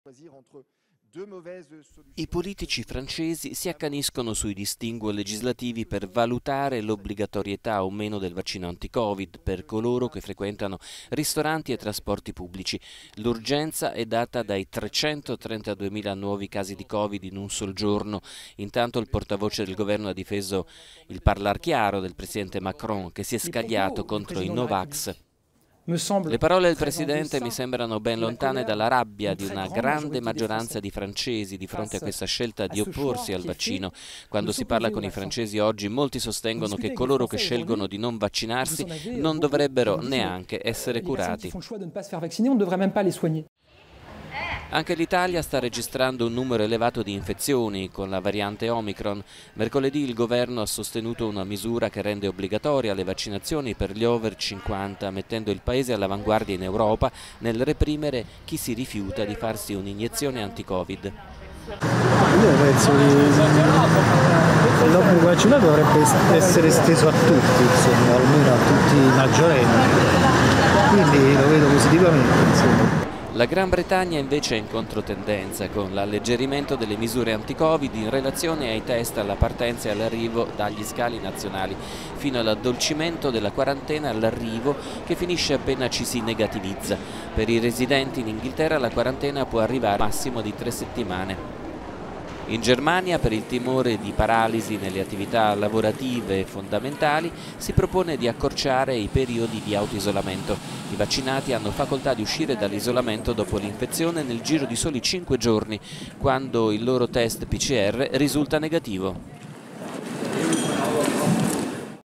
Europa a geometria variabile nella prevenzione del covid
Le accalorate discussioni al parlamento francese, l'Italia che vuole costringere gli over 50 a vaccinarsi, la Germania che alleggerisce la quarantena per non penalizzare le attività lavorative e la Gran Bretagna che riduce i tempi di isolamento: ecco come cambiano le regole